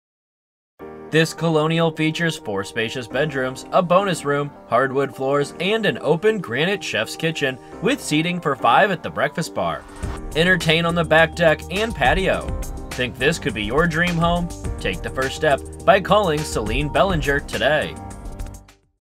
349 New Boston Road Bedford, NH | ColdwellBankerHomes.com
349 New Boston Road, Bedford, NH

2005 colonial set back from the road surrounded by trees for ultimate privacy. Open granite kitchen with breakfast bar that comfortably seats five overlooks the living room. Stainless Steel appliances, including double ovens. Formal dining room, bonus room on first floor perfect for a home office. T